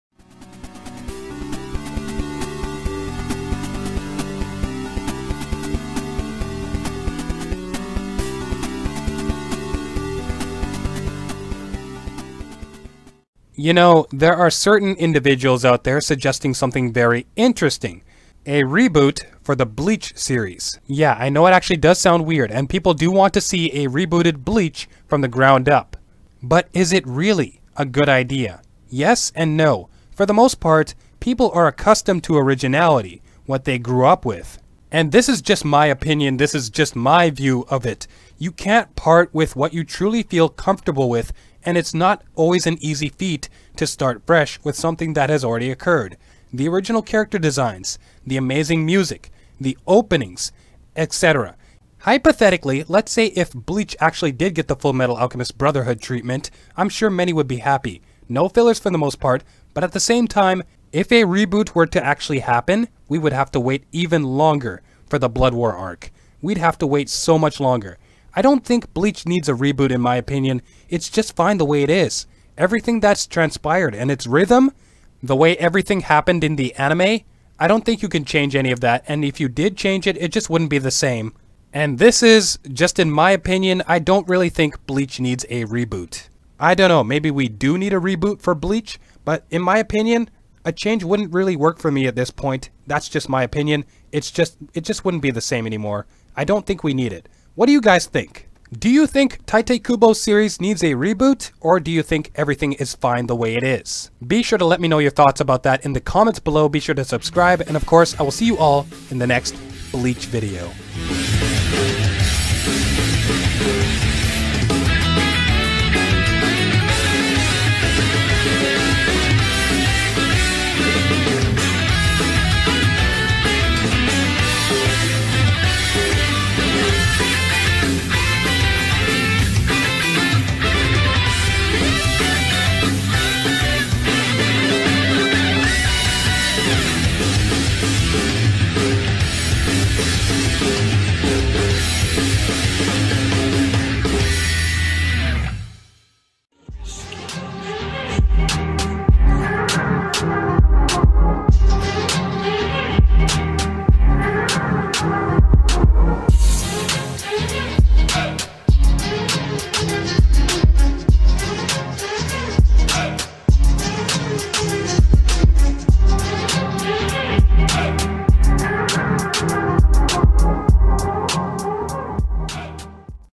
Does BLEACH Even Need A Reboot?
Please don't get it twisted. A Bleach Reboot is NOT happening or anything, but I've seen people suggest this idea and i'm throwing out my honest opinion on this matter. What do you guys think? Should Bleach get a Reboot? Or is it fine the way it is? Would it be a step forward? Or a step backward?